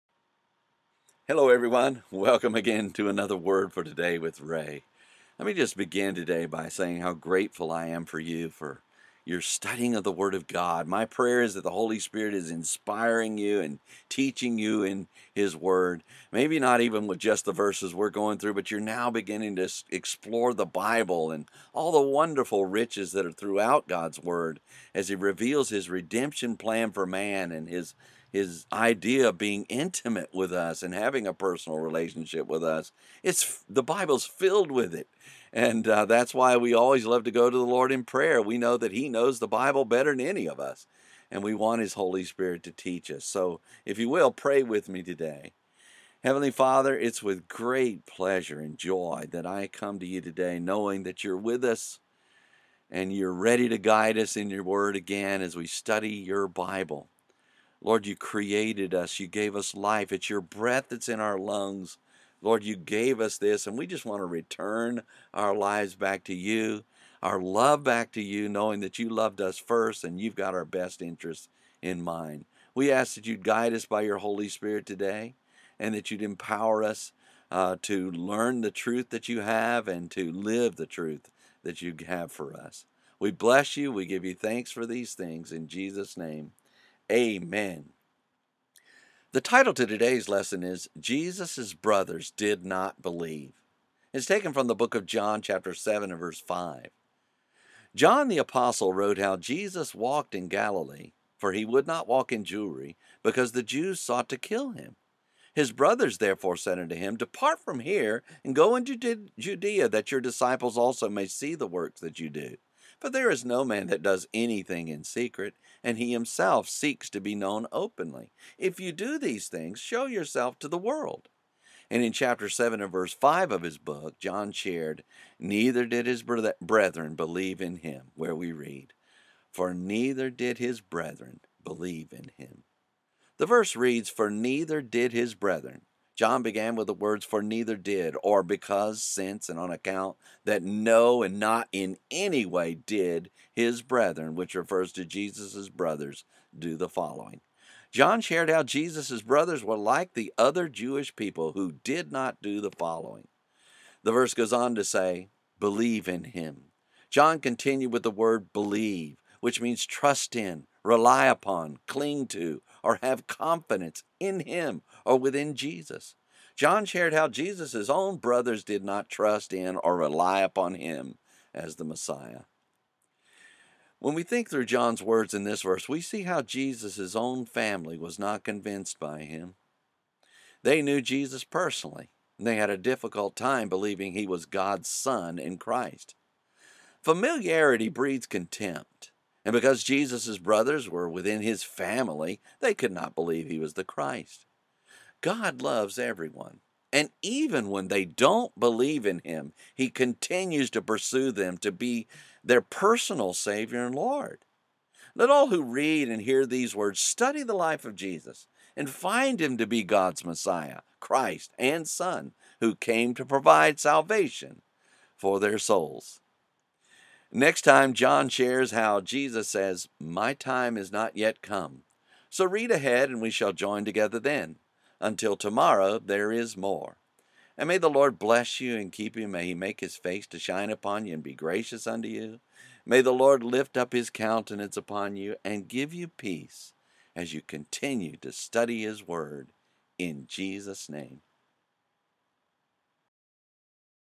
Jesus' Brothers Did Not Believe - John 7:5 (made with Spreaker)
Verse by verse study through the book of John Chapter Seven and Verse Five